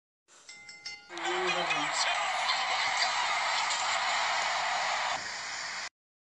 WWE Money In The Bank: UnderTaker Vs Sting Stop Motion Preview
WWE Money In The Bank: UnderTaker Vs Sting Stop Motion Preview. I know it isn't long but I needed to get something out for you guys.